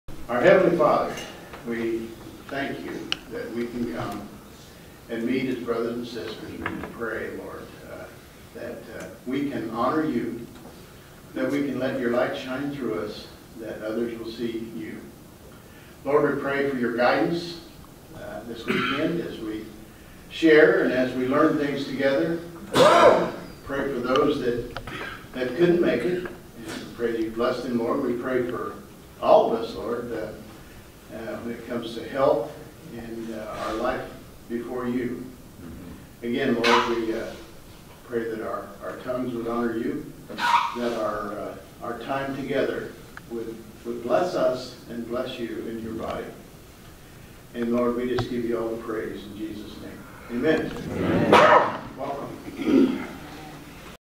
Point Man Opening Prayer
Over the weekend I was in Buffalo at the Point Man International Ministries conference. Steven Williams started it off with a prayer. Pretty much, the whole weekend was about what is possible and how much prayer empowered us to keep this mission going since 1984.